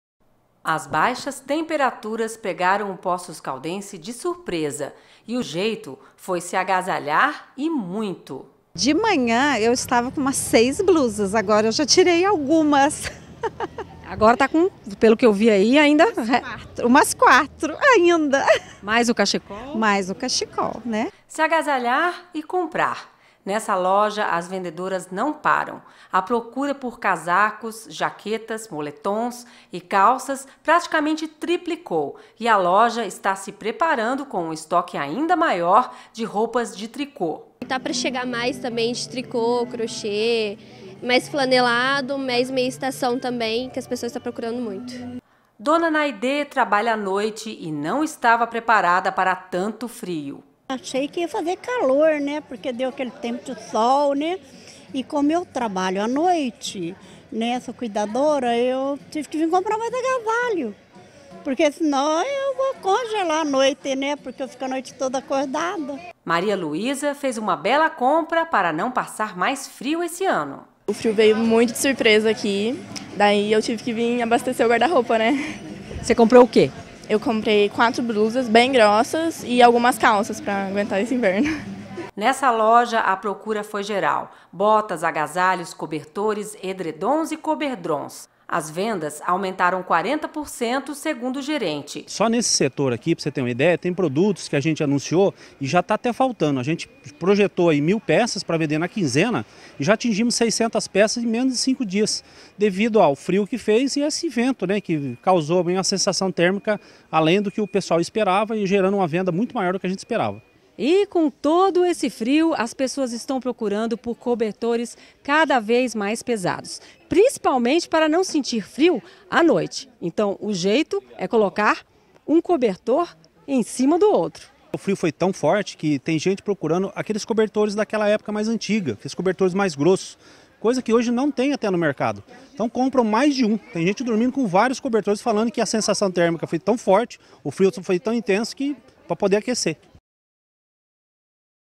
VENDAS DE ROUPAS E COBERTORES
O frio vem batendo recorde nesse inverno e o comércio de Poços triplicou a venda de agasalhos e cobertores.